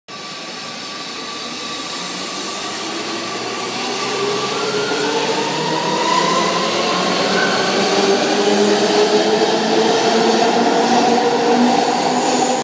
Vertrek GVB 110/109 te Europaplein - Noord/Zuidlijn
GVB Metro M5 110/109 vertrekt als metro M52 naar Amsterdam Noord, vanaf metrostation Europaplein, op de eerste dag van de Noord/Zuidlijn